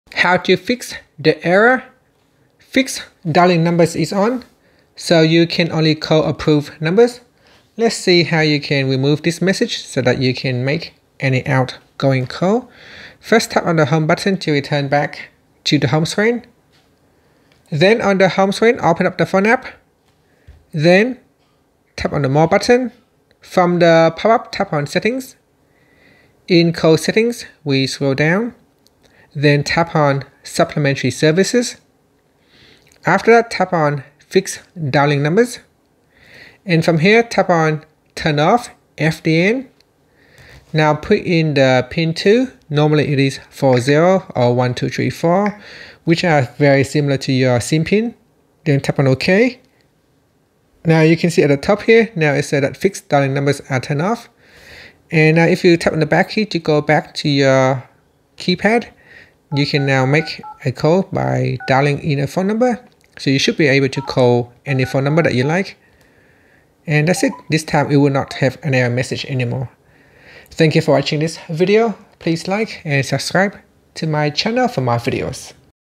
Galaxy S24 Error Fix: "FDN is On, Call Approved Numbers Only" (Fixed Dialing Numbers)
Got the "Fixed Dialing Numbers is on" error on your Galaxy S24/S24+? This quick guide shows you how to turn off FDN and make calls without restrictions. Let's get it fixed!